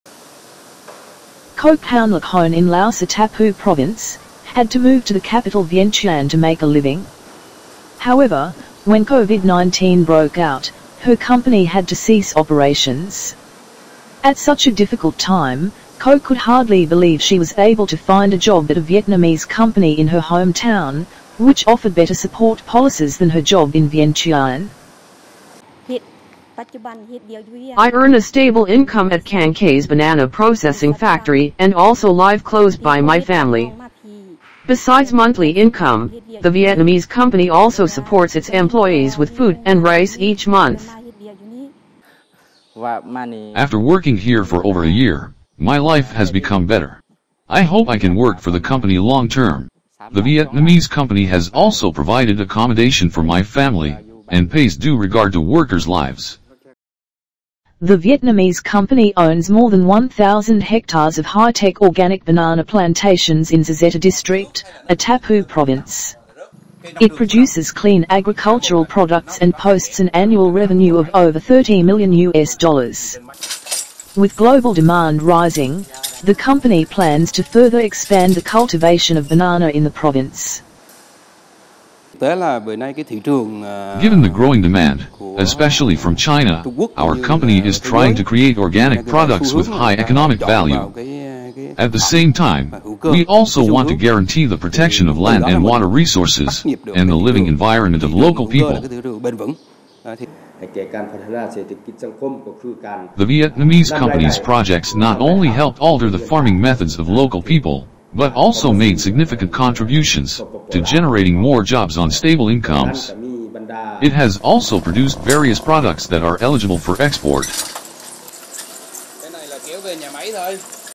Vietnamese firms improving lives of Lao people
The consumption of organic food has become popular in the world. Recognizing this global trend, Vietnamese investors in Laos have been producing organic farm produce, bringing stable incomes for Lao people.